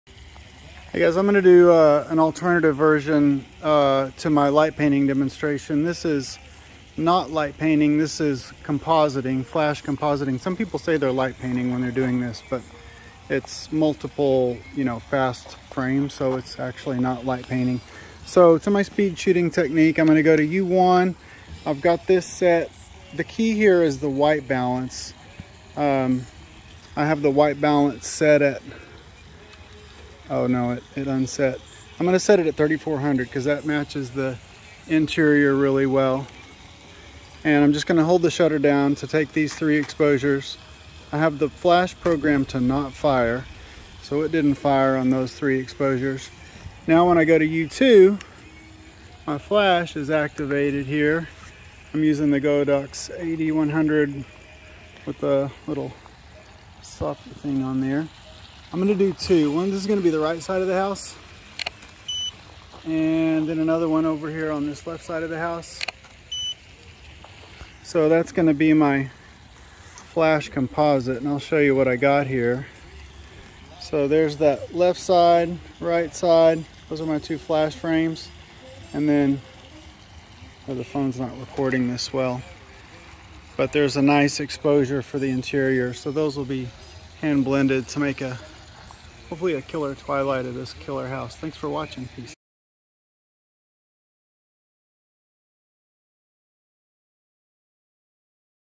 NOT Light Painting Demonstration, twilight real estate photography flash/ambient compositing
Flash compositing technique for twilight real estate exteriors technique using flash.